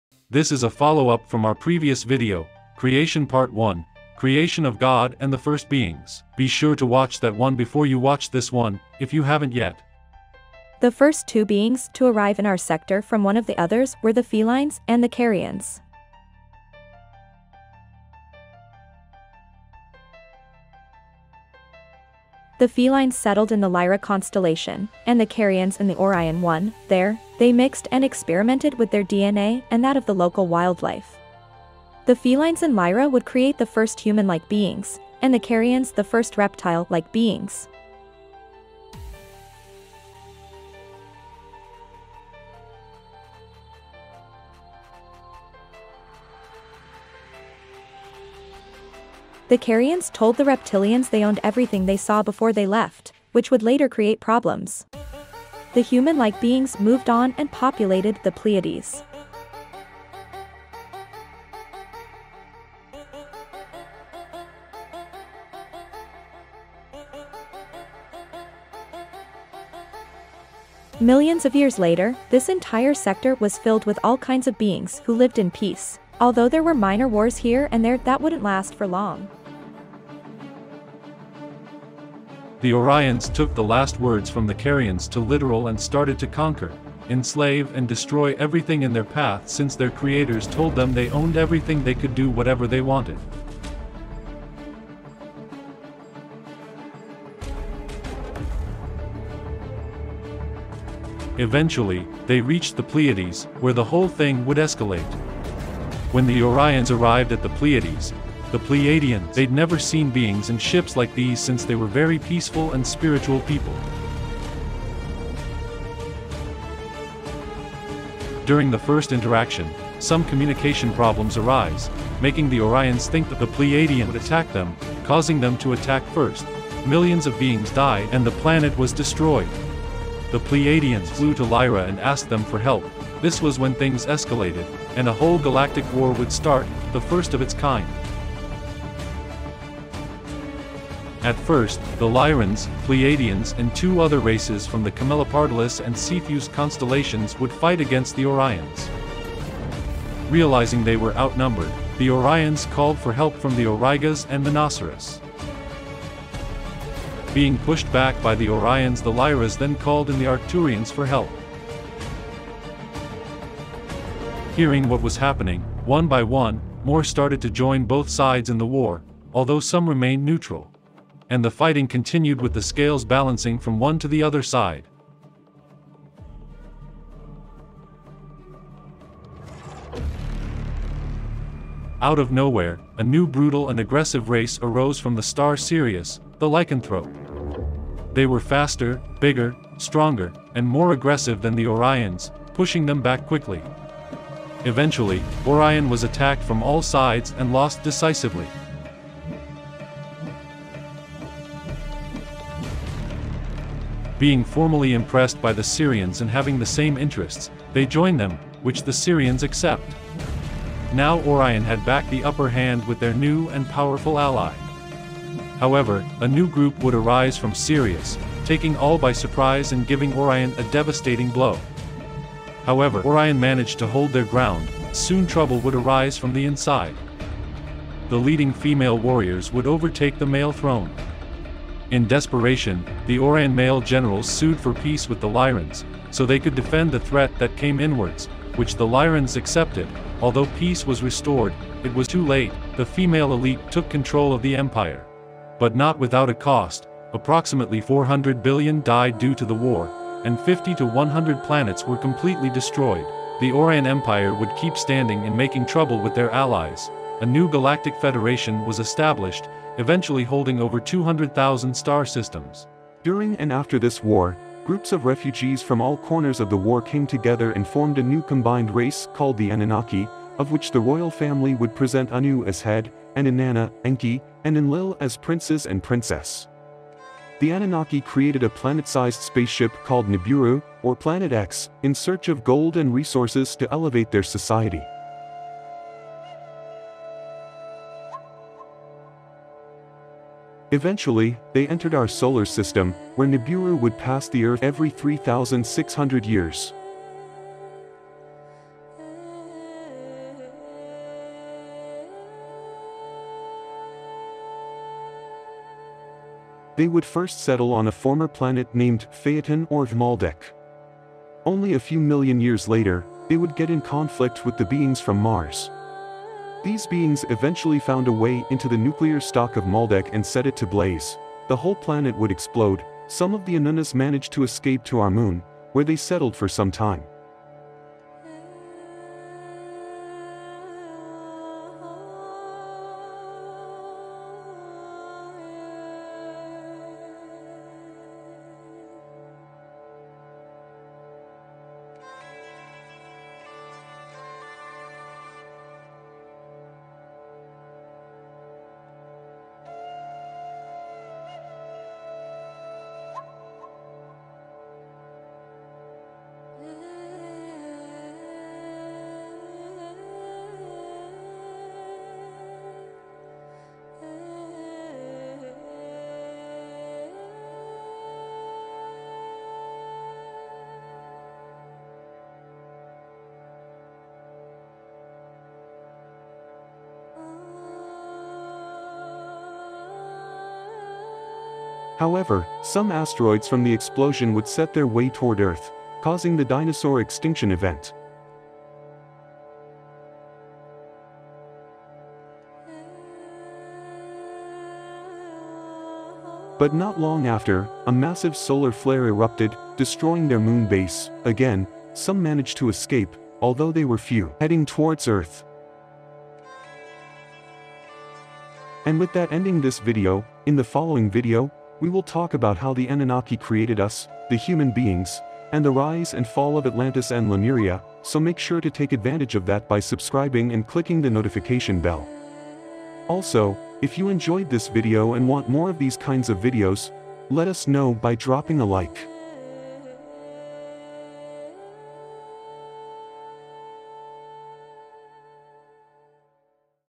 (OLD VERSION) Creation part2 : Galactic war, and the creation of the Anunnaki!
Creation part2 : Galactic war, and the creation of the Anunnaki!

In This video, we talk about, the First Galactic war, and the creation of the Anunnaki.

BTC: 33cCi2vQzAx76gy5XNT2sN5m3QcxRGrpsM
ETH: 0x8D78A2f9e3776bC134E677B15d84493d95E50Be8

May source be with you all!

00:00 Introduction
00:12 the first beings
01:12 Trouble on the horizon
02:21 Galactic war
03:02 Lycanthrope
03:44 Trouble from within
03:57 The Peace
04:08 The aftermath
04:22 The Anunaki
04:44 The Journey
05:20 The war Maldek Martian
05:49 Unrest
06:47 End